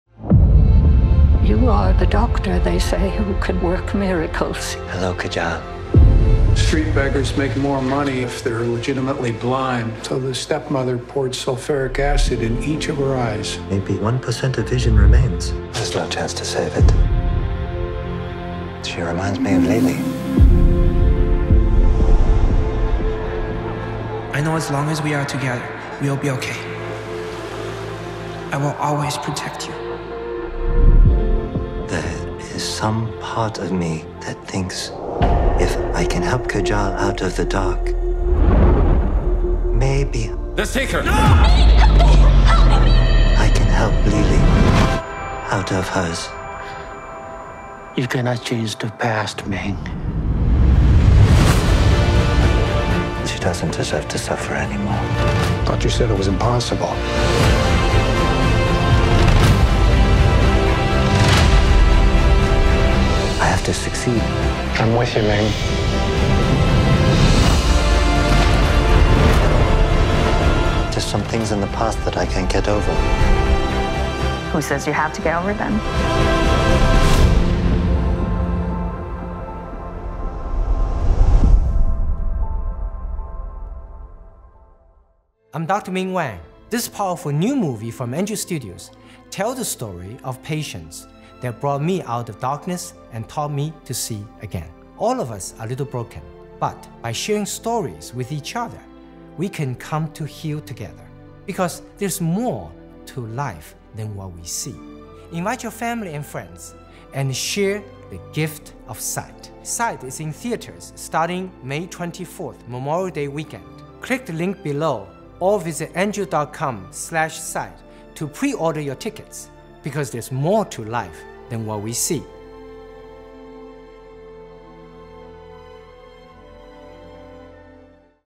Angel Studios Trailer with Dr. Ming Wang, Harvard & MIT (MD, magna cum laude); PhD (laser physics)
Angel Studios Trailer with Dr. Ming Wang, Harvard & MIT (MD, magna cum laude); PhD (laser physics) with CTA